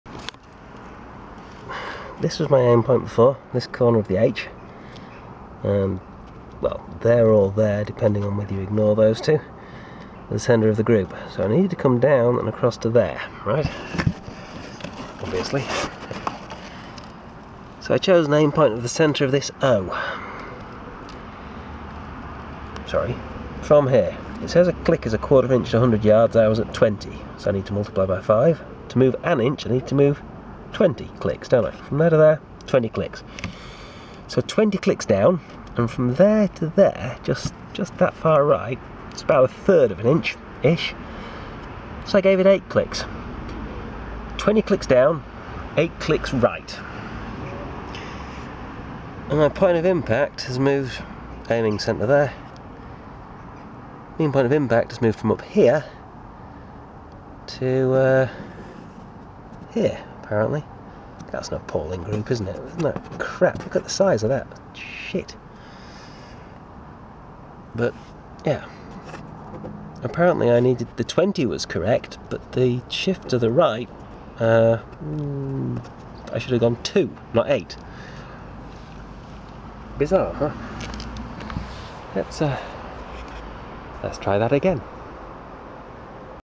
Checking zero on the TX200 part 2
Adjust sights and try again and ... miles out!